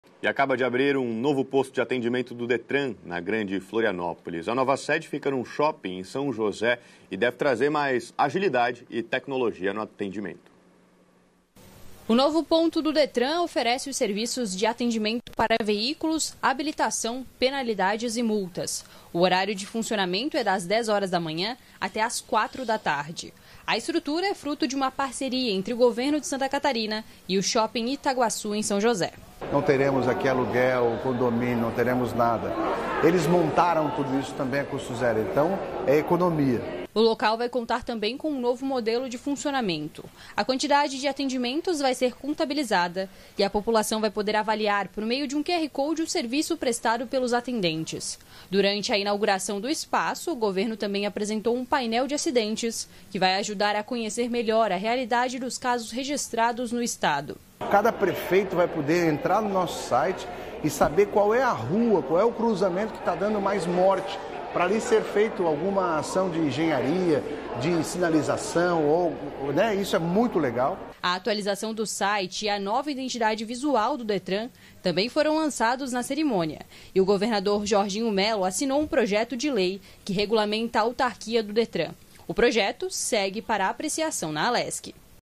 Facilidade e tecnologia: novo posto do Detran abre em shopping na Grande Florianópolis
O Detran acaba de inaugurar um novo posto de atendimento na Grande Florianópolis, localizado no Shopping Itaguaçu, em São José. A iniciativa, fruto da parceria entre o Governo de Santa Catarina e o shopping, promete trazer agilidade e tecnologia aos serviços oferecidos. Entre os benefícios da nova estrutura estão: atendimentos para veículos, habilitação, atrasos e multas, com horário de funcionamento estendido, das 10h às 16h. Saiba mais sobre como essa iniciativa busca facilitar o acesso e melhorar a experiência dos usuários.